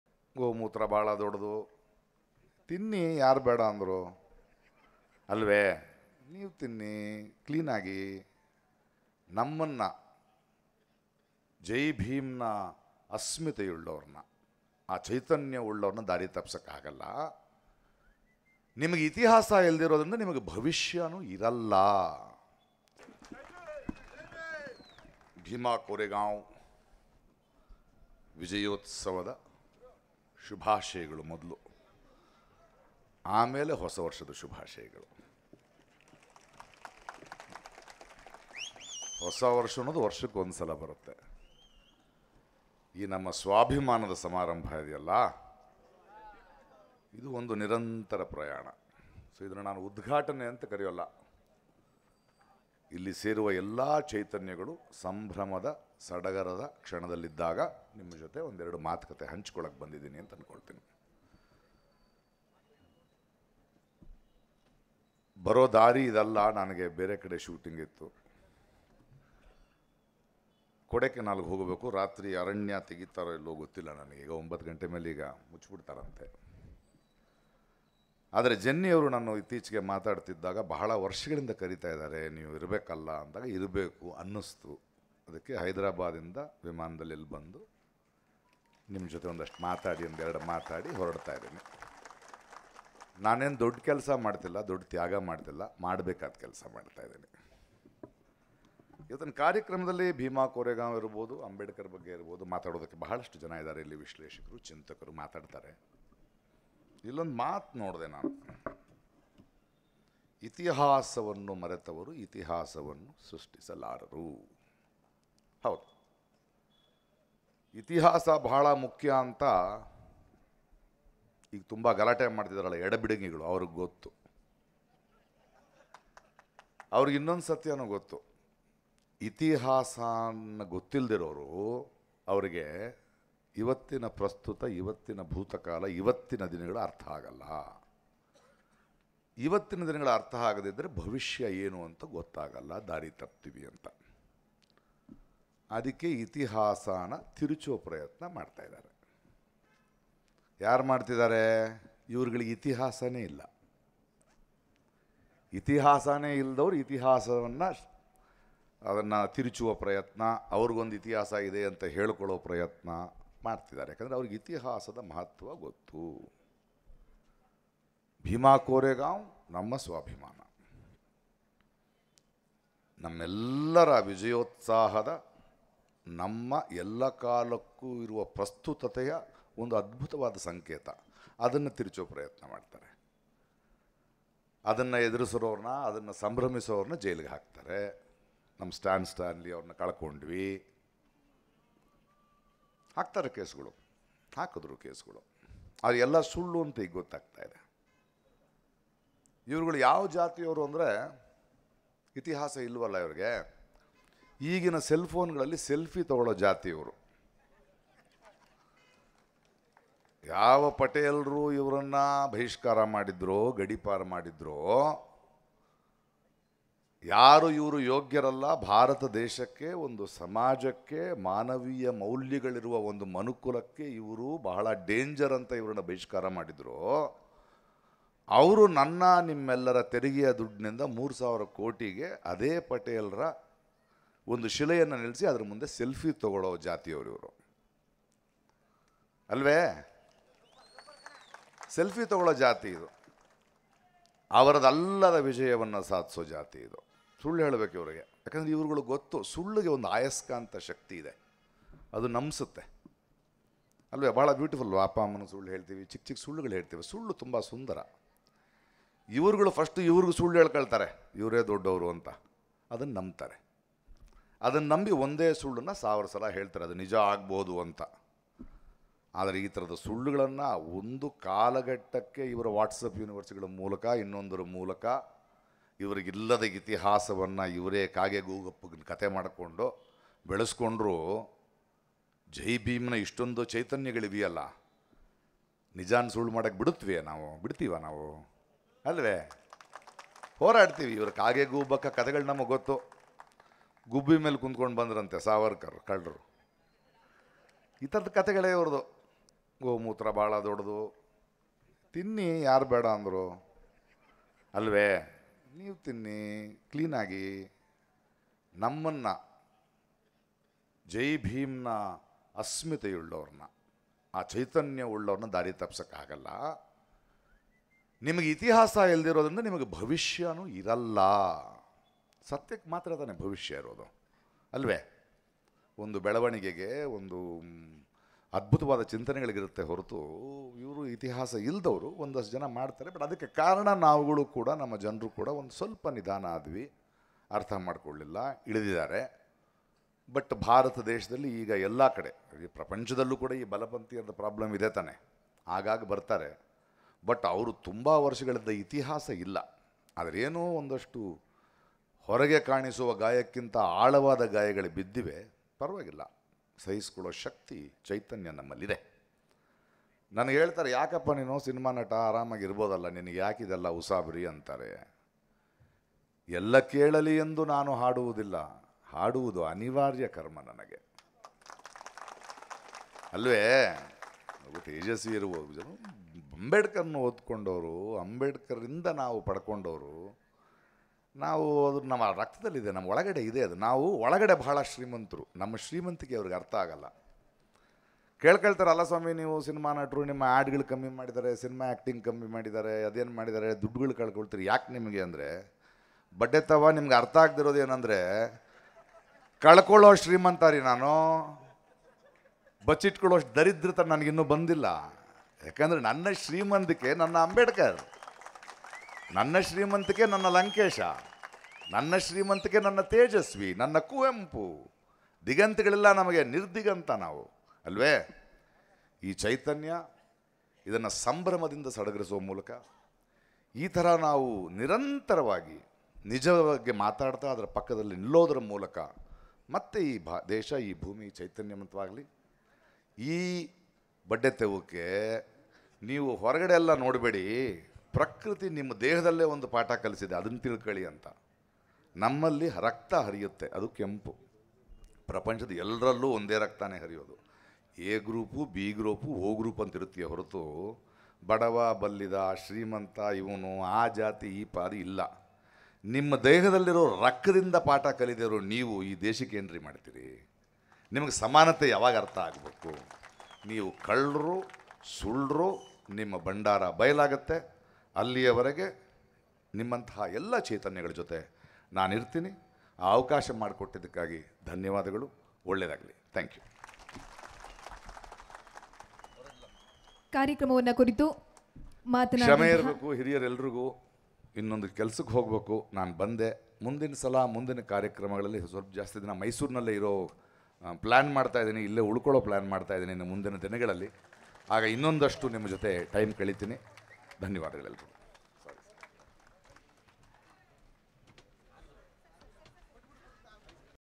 Prakash Rai | ಗೋ ಮೂತ್ರ ಪವಿತ್ರ ಅನ್ನೋರು ತಿನ್ನಿ ಯಾರು ಬೇಡ ಅಂದ್ರು: ಬಹುಭಾಷಾ ನಟ ಪ್ರಕಾಶ್ ರೈ | Mysore
Nandini Mysore

ನಂದಿನಿ ಮೈಸೂರು

ಇತಿಹಾಸವೇ ಇಲ್ಲದವರು ದೇಶದ ಇತಿಹಾಸ ತಿರುಚುವ ಕೆಲಸ ಮಾಡುತ್ತಿದ್ದಾರೆ: ಬಹುಭಾಷಾ ನಟ ಪ್ರಕಾಶ್ ರಾಜ್

ಮೈಸೂರು,ಜ.1: ಇತಿಹಾಸವೇ ಇಲ್ಲದವರು ದೇಶದ  ಇತಿಹಾಸವನ್ನು ತಿರುಚುವ ಕೆಲಸವನ್ನು ಮಾಡುತ್ತಿದ್ದಾರೆ ಎಂದು ಬಹುಭಾಷಾ ನಟ ಪ್ರಕಾಶ್ ರಾಜ್ ಆಕ್ರೋಶ ವ್ಯಕ್ತಪಡಿಸಿದರು.

ಜೈಭೀಮಾ ಕೋರೆಂಗಾವ್ ವಿಜಯೋತ್ಸವ ಸಮಿತಿಯಿಂದ  ಅಶೋಕಪುರಂ ಬಳಿ ಇರುವ ಜಯನಗರ ರೈಲ್ವೆ ಗೇಟ್ ಬಳಿ ಇರುವ ಕೋರೆಂಗಾವ್ ಸ್ಥಂಭಕ್ಕೆ ರವಿವಾರ   ಪುಷ್ಪಾರ್ಚನೆ ಮಾಡಿ ಭೀಮಾ ಕೋರೆಗಾಂವ್ 205ನೇ ವಾರ್ಷಿಕೋತ್ಸವ ಉದ್ಘಾಟಿಸಿ ಮಾತನಾಡಿದರು.
ಎಡಬಿಡಂಗಿಗಳಿಗೆ ಇತಿಹಾಸವಿಲ್ಲ, ಇತಿಹಾಸವೂ ಗೊತ್ತಿಲ್ಲ. ಇದರಿಂದಲೇ ಇವರಿಗೆ ಭೂತಕಾಲ, ವರ್ತಮಾನದ ಕಾಲವೂ ಅರ್ಥವಾಗುತ್ತಿಲ್ಲ. ಭವಿಷ್ಯತ್ ಕಾಲದ ಕುರಿತು ಅರಿವಿಲ್ಲ. ಅದಕ್ಕಾಗಿ ದಾರಿ ತಪ್ಪಿಸುವ ಕೆಲಸವನ್ನು ಮಾಡುತ್ತಿದ್ದಾರೆ. ಹಾಗಾಗಿ  ಅವರಿಗೆ ಅನುಕೂಲಕರವಾಗುವಂತೆ ಇತಿಹಾಸವನ್ನು ಬದಲಿಸಲು ಹೊರಟಿದಿದ್ದಾರೆ ಎಂದು ಕಿಡಿಕಾರಿದರು.
ಭೀಮಾ ಕೋರೆಂಗಾವ್  ವಿಜಯೋತ್ಸವ ದೇಶದ ಸ್ವಾಭಿಮಾನದ ಸಂಕೇತ. ಇದು ಜೈಭೀಮ್ ಅನುಯಾಯಿಗಳ ಅಸ್ಮಿತೆ ಮತ್ತು ಅಸ್ತಿತ್ವ. ಇದನ್ನು ಸಹಿಸಿಕೊಳ್ಳದ ಮನಸ್ಸುಗಳು ಈ ಚರಿತ್ರೆಯನ್ನು ತಿದ್ದಲು ಹುನ್ನಾರ ನಡೆಸಿವೆ. ಆದರೆ, ಇದು ಸಾಧ್ಯವಿಲ್ಲ. ನಮಗೆಲ್ಲ ಇದೆಲ್ಲ ನಿಧಾನವಾಗಿ ಅರ್ಥವಾಗುತ್ತಿದೆ ಎಂದರು.
ಭೀಮಾ ಕೊರೆಗಾಂವ್ ಹಿಂಸಾಚಾರ ಪ್ರಕರಣಕ್ಕೆ ಸಂಬಂಧಿಸಿದಂತೆ ಮಾನವ ಹಕ್ಕುಗಳ ಹೋರಾಟಗಾರ ಫಾದರ್ ಸ್ಟ್ಯಾನ್ ಸ್ವಾಮಿ ವಿರುದ್ಧ ಸುಳ್ಳು ಪ್ರಕರಣ ದಾಖಲಿಸಲಾಯಿತು. ಹೀಗೆ ಅನೇಕರ ವಿರುದ್ಧವೂ ಎಫ್‌ಐಆರ್ ಹಾಕಲಾಗಿದೆ. ಆದರೀಗ ಇವುಗಳೆಲ್ಲ ‘ಸುಳ್ಳು ಕೇಸ್’ ಎಂಬುದು ಗೊತ್ತಾಗಿದೆ. ಇದೆಲ್ಲ ಇತಿಹಾಸ ಗೊತ್ತಿಲ್ಲದವರು ಮಾಡುವ ಕುತಂತ್ರ ಎಂದು ಆರೋಪಿಸಿದರು.
ಇವರು ಸೇಲ್‌ಫೋನ್‌ನಲ್ಲಿ ಸೆಲ್ಫಿ ತೆಗೆದುಕೊಳ್ಳುವ ಜಾತಿಯವರು. ಈ ಹಿಂದೆ ಇವರನ್ನು ಪಟೇಲರು ಗಡಿಪಾರು ಮಾಡಿದ್ದರು. ಮನುಕುಲ ಸಮಾಜದಲ್ಲಿ ಇರಲು ಯೋಗ್ಯರಲ್ಲದವರು ಎಂದು ಹೀಗೆ ಮಾಡಲಾಗಿತ್ತು ಎಂದರು.
ಸುಳ್ಳಿಗೆ ಆಯಸ್ಕಾಂತೀಯ ಶಕ್ತಿ ಇದೆ. ಜನರನ್ನು ದಾರಿ ತಪ್ಪಿಸಲು ಸುಳ್ಳಿನ ಸರಮಾಲೆಯನ್ನೇ ಬಿತ್ತಲಾಗುತ್ತಿದೆ. ವ್ಯಾಟ್ಸ್‌ಆ್ಯಪ್ ಯೂನಿರ್ವಸಿಯ ಮೂಲಕ ಕಾಗಕ್ಕ ಗುಬ್ಬಕ್ಕನ ಕಥೆಯಂತಹ ಇತಿಹಾಸವನ್ನು ಸೃಷ್ಟಿ ಹರಿಬಿಡಲಾಗುತ್ತಿದೆ. ಬುಲ್‌ಬುಲ್ ಹಕ್ಕಿ ಮೇಲೆ ಸಾವರ್ಕರ್ ಪ್ರಯಾಣ, ಗೋಮೂತ್ರ ಸೇವನೆ ಆರೋಗ್ಯಕರ ಎಂಬ ಹಸಿಸುಳ್ಳು ಇದಕ್ಕೆ ಇತ್ತೀಚಿನ ನಿದರ್ಶನವಾಗಿದೆ. ಗೋಮೂತ್ರ ಶ್ರೇಷ್ಠವಾಗಿದ್ದರೆ ನೀವು ಕುಡಿಯಿರಿ. ನಾವು ಸ್ವಚ್ಛವಾಗಿ ಇರಲು ಬಿಡಿ ಎಂದರು.
ಬಲಪಂಥೀಯರ ಎಡಬಿಡಂಗಿತ ಪ್ರಪಂಚಾದ್ಯಂತ ಎಲ್ಲೆಡೆ ಇದೆ. ಇವರ ಆಟವೆಲ್ಲ ಕೆಲ ವರ್ಷಗಳು ಮಾತ್ರ ನಡೆಯುತ್ತದೆ. ನಿಜಾಂಶಗೊತ್ತಾದ ಬಳಿಕ ಇದೆಲ್ಲ ನಡೆಯಲ್ಲ. ಇವರು ಸೃಷ್ಟಿಸುವ ಗಾಯವನ್ನು ತಡೆದುಕೊಂಡು, ಸುಧಾರಿಸಿಕೊಳ್ಳುವ ಶಕ್ತಿ ಈ ದೇಶಕ್ಕೆ ಇದೆ ಎಂದರು.
‘ಯಾರಿಗೂ ಇಲ್ಲದ ಉಸಾಬರಿ ನಿನಗೆ ಏಕೆ? ಸುಮ್ಮನೆ ಸಿನಿಮಾ ಮಾಡಿಕೊಂಡು ಹೋಗು’ ಎಂದು ಅನೇಕರು ನನಗೆ ಸಲಹೆ ನೀಡುತ್ತಾರೆ. ನಟನೆ ನನ್ನ ಕರ್ಮ. ಅದು ಮುಂದುವರಿಯುತ್ತದೆ. ನಾವುಗಳೆಲ್ಲ ಅಂಬೇಡ್ಕರ್, ಕುವೆಂಪು, ಲಂಕೇಶ್, ಪೂರ್ಣಚಂದ್ರ ತೇಜಸ್ವಿ ಅವರಿಂದ ಅರಿವು ಪಡೆದುಕೊಂಡವರು. ಹೃದಯ ಶ್ರೀಮಂತರು. ಕಳೆದುಕೊಳ್ಳುವುದರಲ್ಲಿ ಶ್ರೀಮಂತರು. ಬಚ್ಚಿಡುವವರಲ್ಲ ಎಂದು ಮಾರ್ಮಿಕವಾಗಿ ಹೇಳಿದರು.
ಬೇಧಭಾವ ಮಾಡದ ಪ್ರಕೃತಿಯಿಂದಲೂ ಬಲಪಂಥೀಯರು ಪಾಠ ಕಲಿಯುತ್ತಿಲ್ಲ. ಇದನ್ನೂ ಅರ್ಥಮಾಡಿಕೊಳ್ಳುತ್ತಿಲ್ಲ. ಇದರ ಮುಂದೆ ಯಾರೂ ಶ್ರೀಮಂತರಲ್ಲ, ಬಡವರಲ್ಲ. ಆ ಜಾತಿ- ಈ ಜಾತಿಯೂ ಇಲ್ಲ. ಎಲ್ಲರೂ ಒಂದೇ. ರಕ್ತದಲ್ಲೂ ಯಾವುದೇ ತಾರತಮ್ಯವಿಲ್ಲ. ಎಲ್ಲರಲ್ಲಿ ಇರುವುದು ಒಂದೇ ರೀತಿಯ ರಕ್ತವೇ. ಇವರೆಲ್ಲ ದೇಹದಲ್ಲಿ ಹರಿಯುವ ರಕ್ತದಿಂದಲೂ ಪಾಠ ಕಲಿಯುತ್ತಿಲ್ಲ ಎಂದು ಕುಟುಕಿದರು.
ಕಾರ್ಯಕ್ರಮದಲ್ಲಿ ಬಂತೇಜಿ, ಸಾಹಿತಿ ಪ್ರೊ.ಕಾಳೇಗೌಡ ನಾಗವಾರ, ವಿಮರ್ಶಕ ಪ್ರೊ.ಓ.ಎಲ್.ನಾಗಭೂಷಣಸ್ವಾಮಿ,  ಮಾಜಿ ಮೇಯರ್ ಪುರುಷೋತ್ತಮ್, ಪಾಲಿಕೆ ಸದಸ್ಯೆ ಪಲ್ಲವಿಬೇಗಂ, ಮೈಸೂರು ವಕೀಲರ ಸಂಘದ ಅಧ್ಯಕ್ಷ ಮಹದೇವಸ್ವಾಮಿ, ಕಾರ್ಯದರ್ಶಿ ಉಮೇಶ್ ನಿವೃತ್ತ ಪೊಲೀಸ್ ಅಧಿಕಾರಿ ಸಿದ್ದರಾಜು, ಭೀಮಾ ಕೋರೆಂಗಾವ್ ವಿಜಯೋತ್ಸವ ಸಮಿತಿಯ ಜಯರಾಜ್, ರವಿ, ದಿಲೀಪ್ ಸೇರಿದಂತೆ ಹಲವರು ಉಪಸ್ಥಿತರಿದ್ದರು.

ಬಿಜೆಪಿಯವರು ಕಳ್ಳರು, ಸುಳ್ಳರು. ಇವರೆಲ್ಲರ ಬಂಡವಾಳ ಇದೀಗ ಬಯಲು ಆಗುತ್ತಿದೆ.
               ‌‌‌‌‌‌                            - ಪ್ರಕಾಶ್ ರಾಜ್,
                                             ಬಹುಭಾಷ ನಟ.

ಅಸಮಾನತೆಯಿಂದ ಬದುಕುತ್ತಿದ್ದ ಕಾಲದಲ್ಲಿ ಸಮಾನತೆ ಬೇಕು, ಸಮಾನತೆ ಬೇಕೆಂದರೆ ಅಧಿಕಾರ ಬೇಕು. ಎಂಬ ಜವಾಬ್ದಾರಿಯ ಎಚ್ಚರಿಕೆ ಮೂಡಿಸಿದವರು ಬಾಬಾಸಾಹೇಬ್ ಅಂಬೇಡ್ಕರ್.
                                     - ಓ.ಎಲ್.ನಾಗಭೂಷಣಸ್ವಾಮಿ,
                                          ವಿಮರ್ಶಕ.